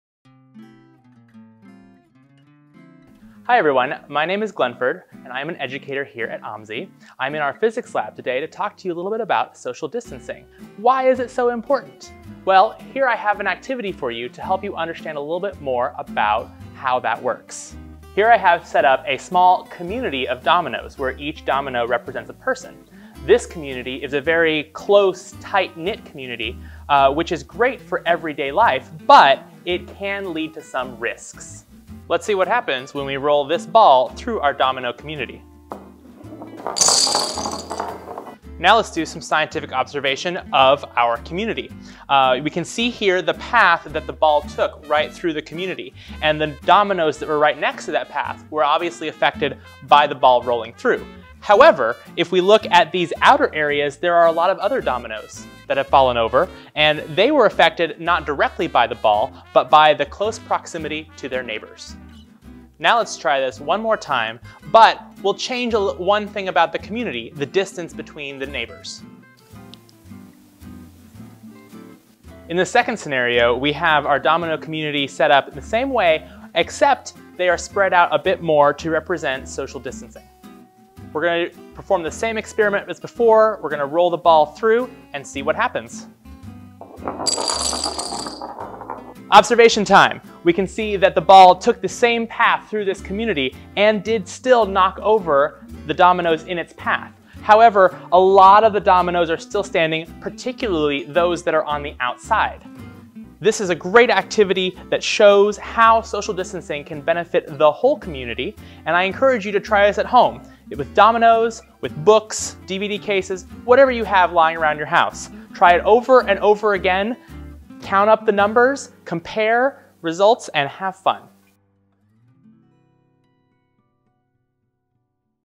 Why is Social Distancing Important?
Join Glenford, one of our science educators, as he shows us a fun experiment you can do at home to learn about the importance of social distancing.